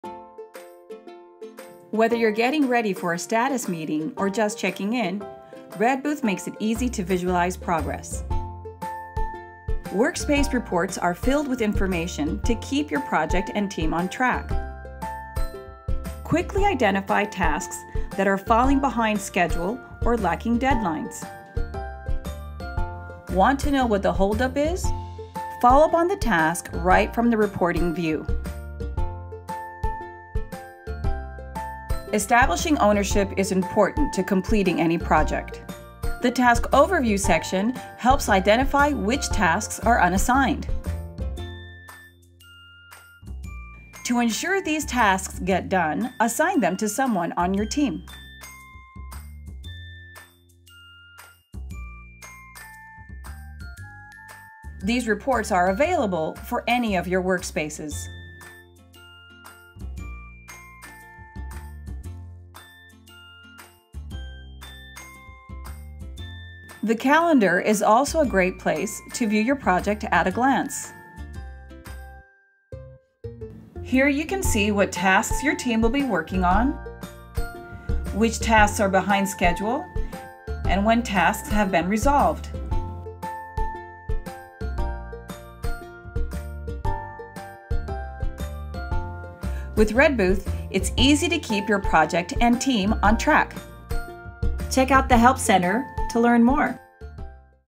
How to Track Your Projects in Redbooth
Learn how Redbooth's reporting features help you keep track of the progress of your projects.